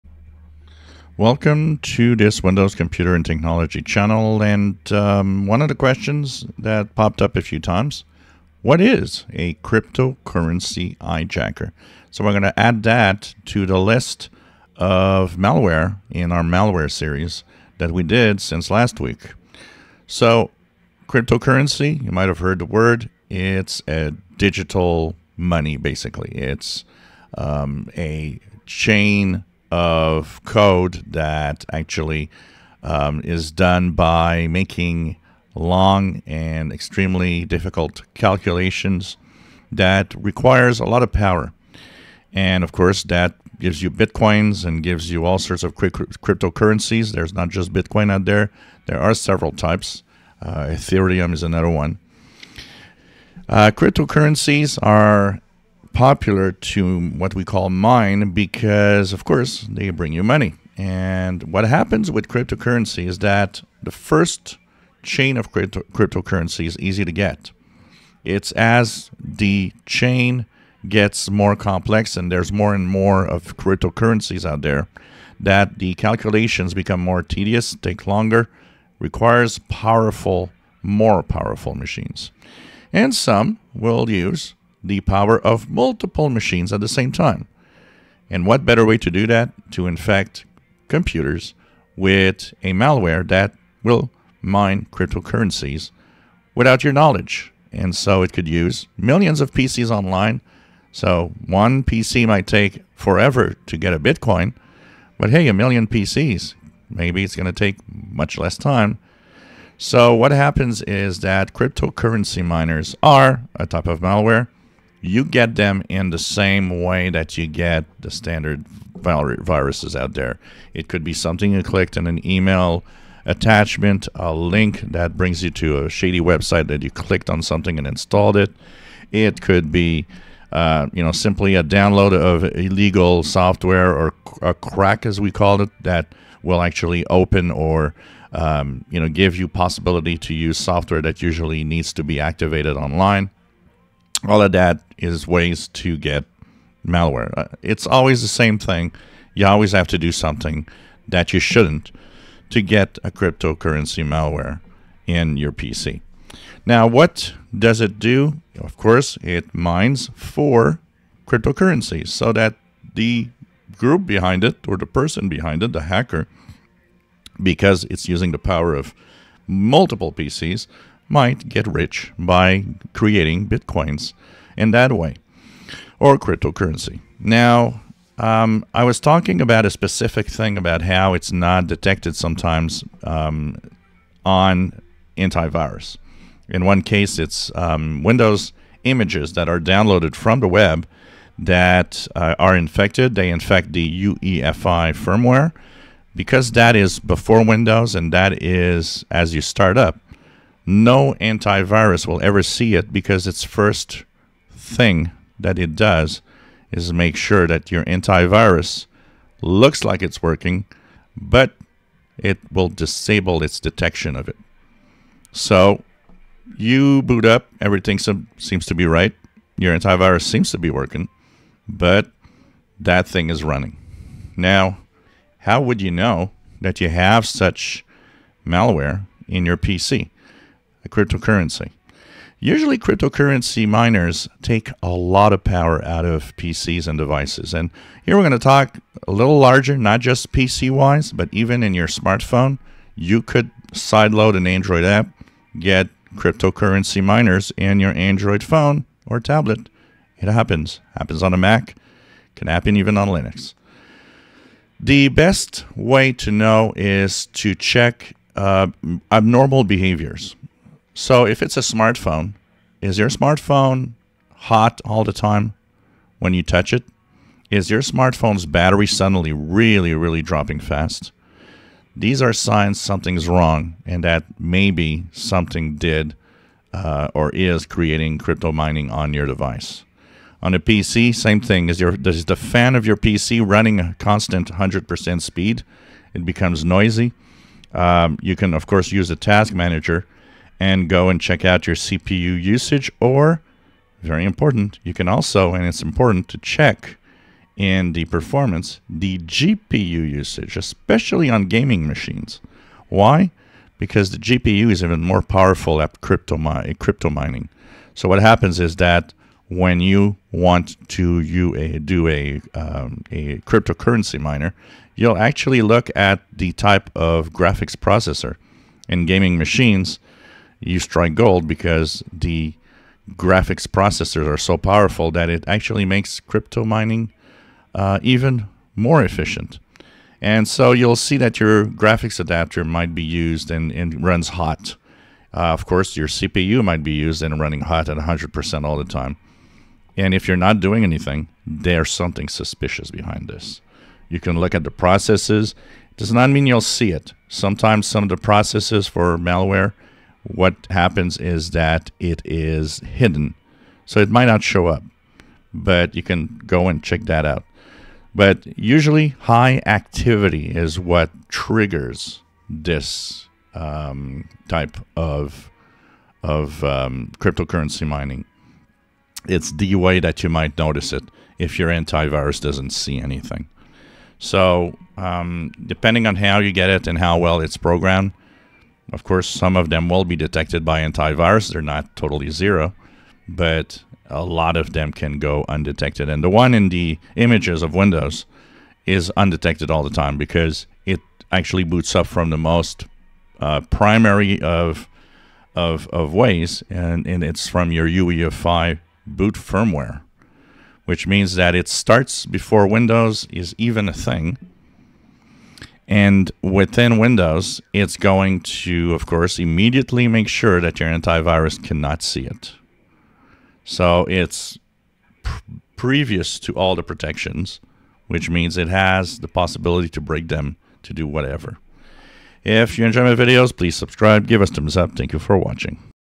CRYPTOCURRENCY Highjack How to know if your PC or Smart phone Tablet might be infected
What is it and How do you detect it if your Antivirus does not